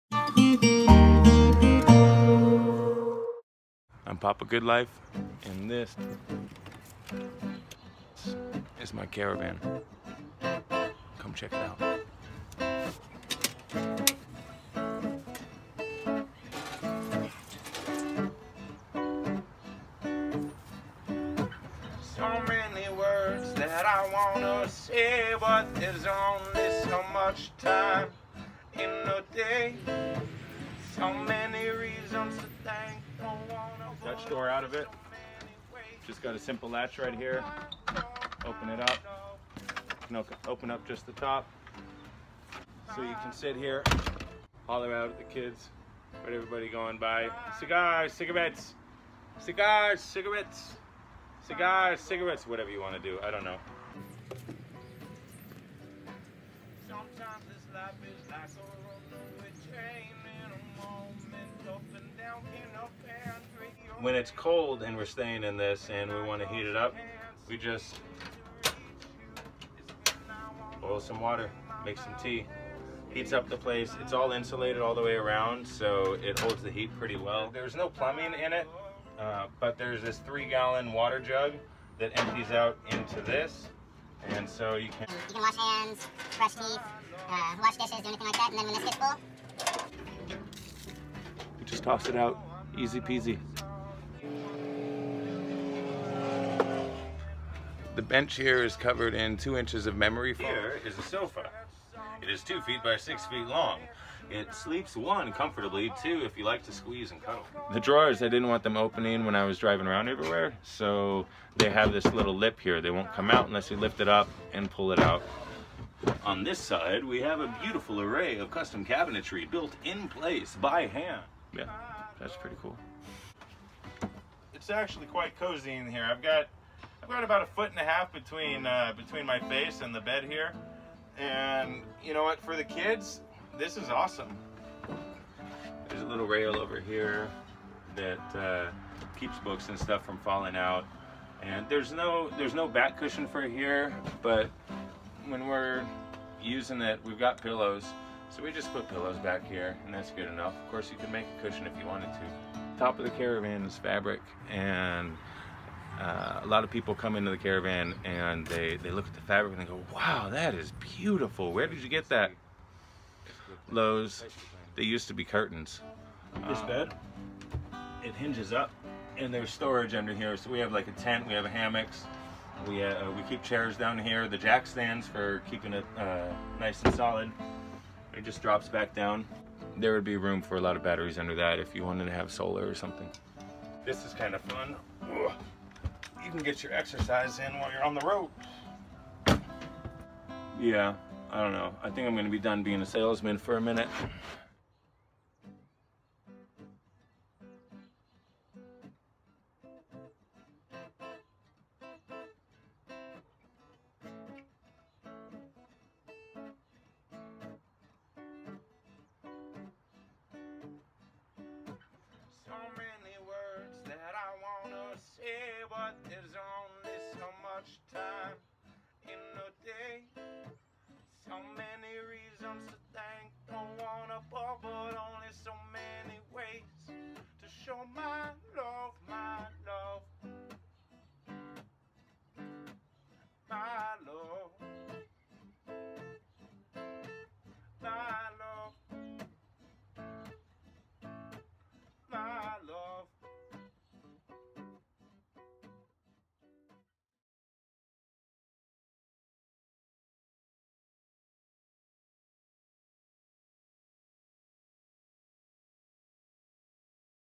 Life In A Gypsy Wagon
"When I build something, I build it as if it is meant for me and my family, as all the custom fluted trimwork and design choices express. I tried to stay true to classic vardo design, while putting my style and personality into it as well, and providing for other challanges such as reduced weight (1660lbs total, including trailer with 1650 lbs capacity) and increased insulation (R-13 in floor, 1/2" rigid foam everywhere else). The synthetic canvas top is waterproof and fire resistant. The construction is cedar and pine exterior (primer and three coats of high quality exterior paint), and all pine and plywood on the interior. The dutch door was custom built from two salvaged doors, and the windows are likewise salvaged."

"I built this Roma (gypsy) caravan styled travel trailer by hand. When this one sells, I’ll build another, or maybe a camper van, a housebus, a housetruck, or a tiny house. Who knows?

I love creating—bringing dreams to life. Buy our caravan and treat yourself to the ultimate glamping mobile!

Delivery in the US may be available. "